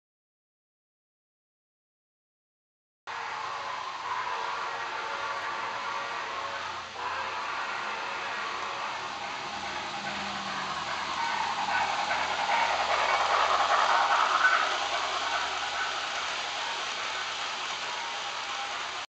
Union Pacific #840 with sound hustles the Overland Limited past Redwood depot
With a virtually matched consist of mostly heavyweight cars, Union Pacific FEF 4-8-4 #840 speeds past Redwood depot with the legendary Overland Limited.